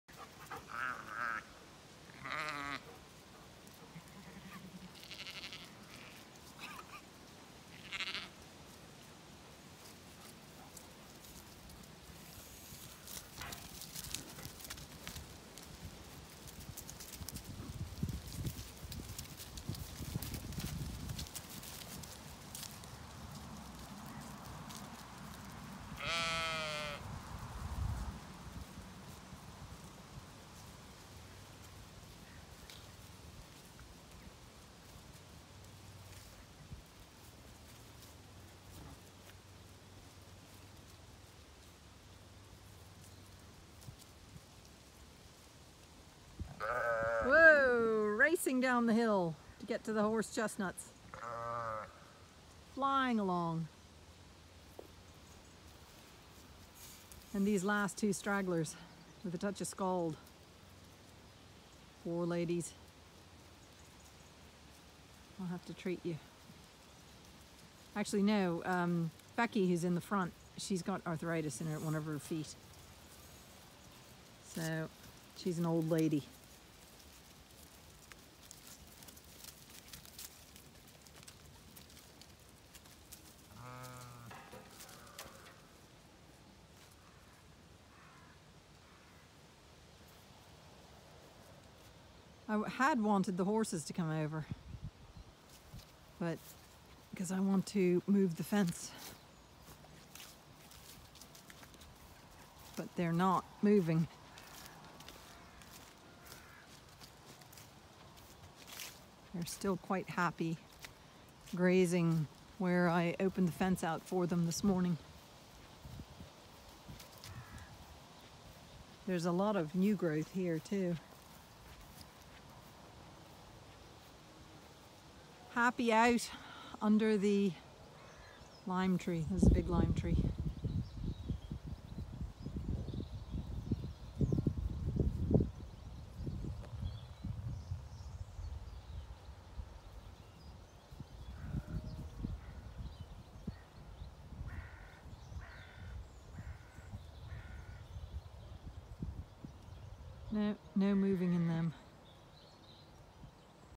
Ewes heard the gate but horses didn’t
I wanted the horses to move but they ignored me while the sheep were ready for more chestnut hunting.